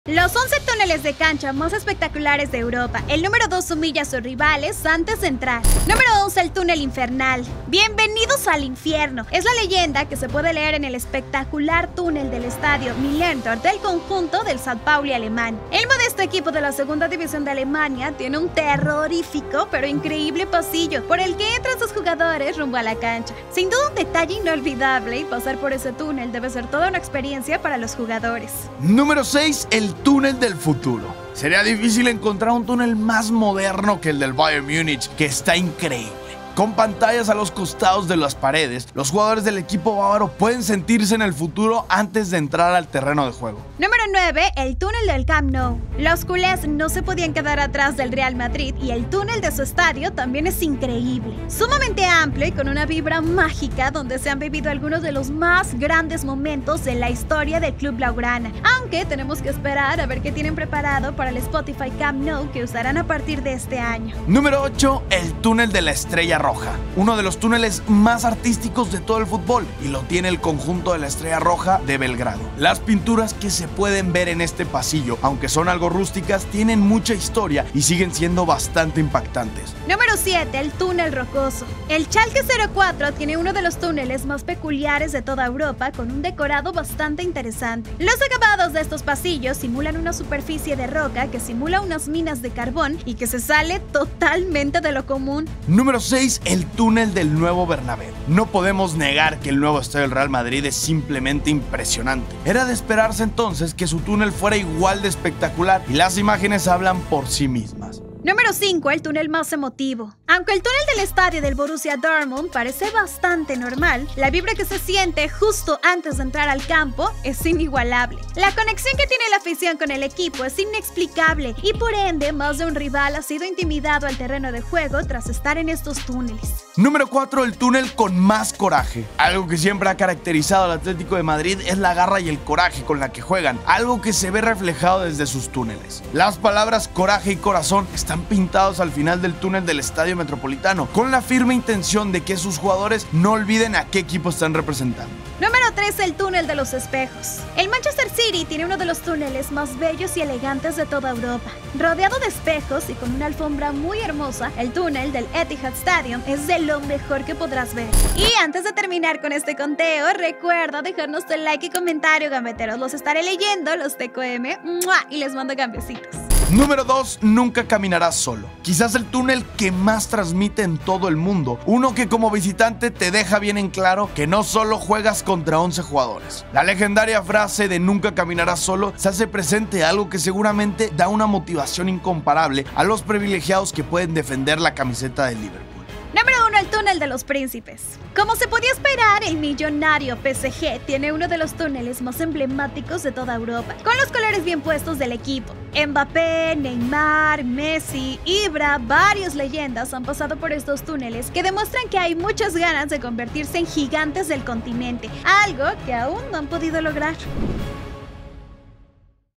¡Los 11 TÚNELES DE CANCHA MÁS ESPECTACULARES de EUROPA!¡EL #2 HUMILLA a sus RIVALES ANTES de ENTRAR!
Los túneles que te hacen soñar en los estadios de Europa.